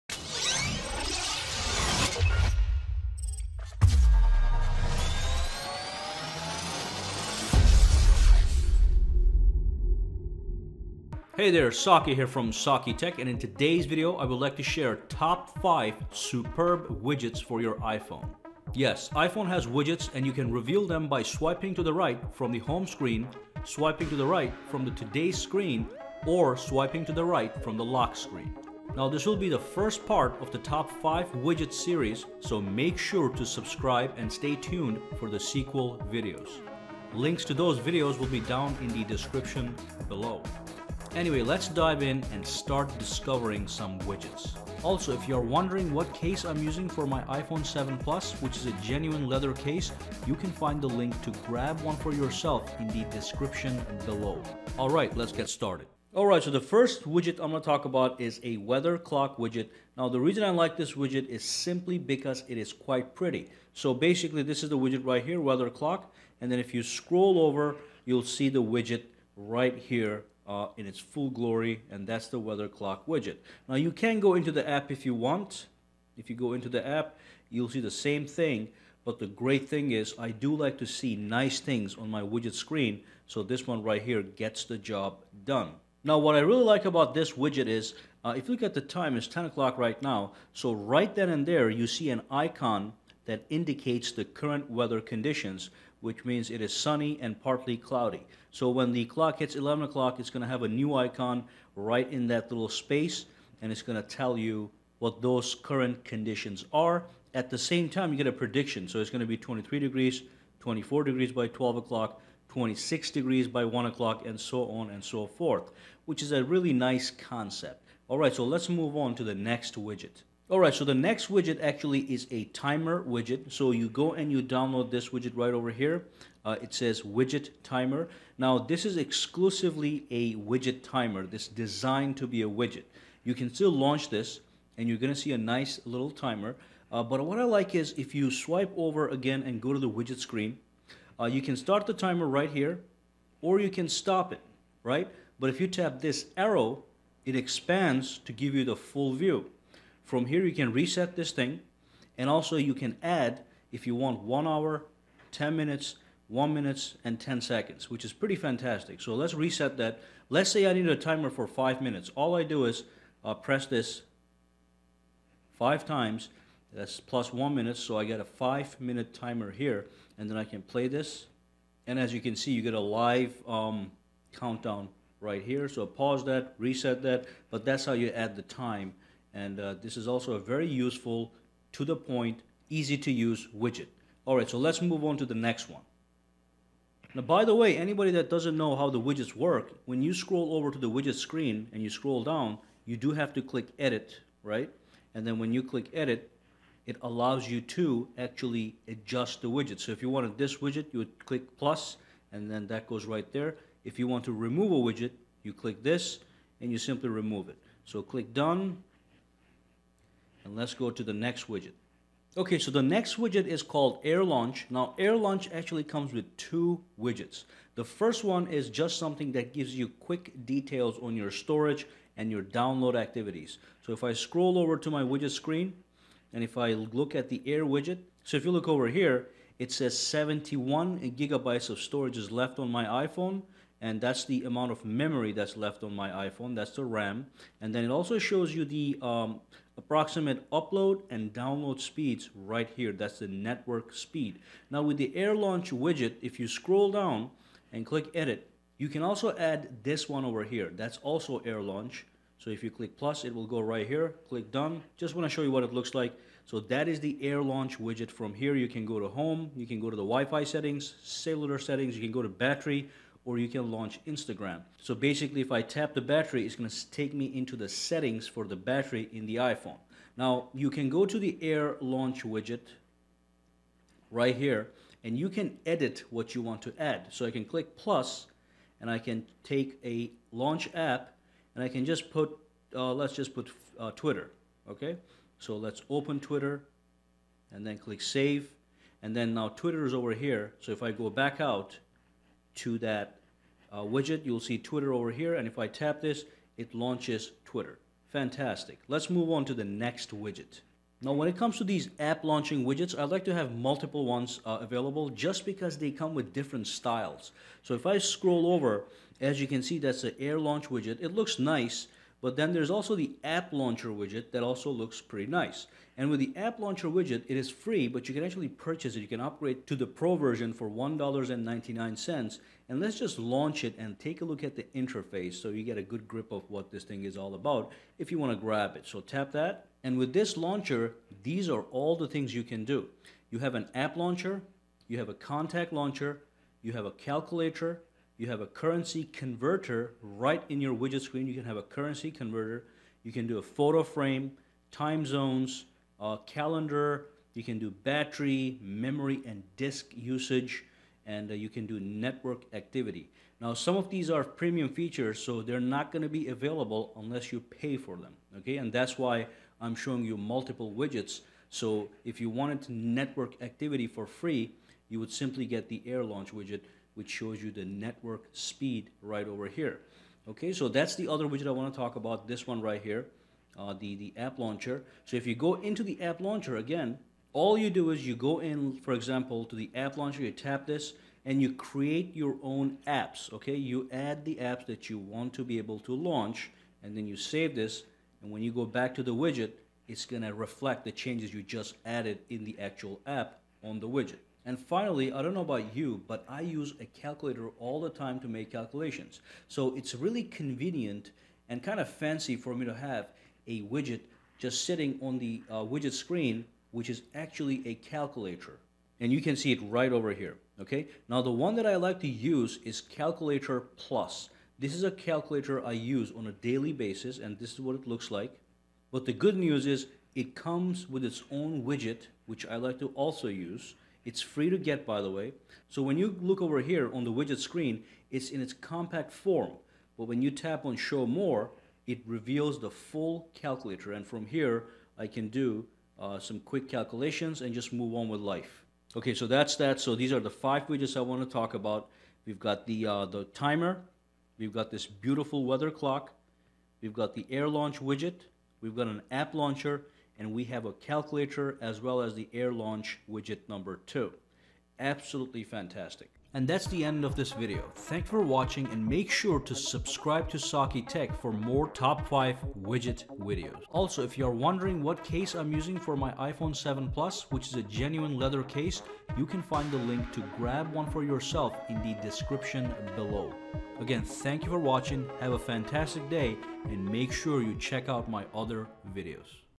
Top 5 Widget Apps for your iPhone for 2017
In this video, we will share top 5 widgets apps for your iPhone 7 Plus. Widgets are apps you download that have widget capabilities. Some of these widgets apps have gorgeous interfaces and very useful features. Let's dive in.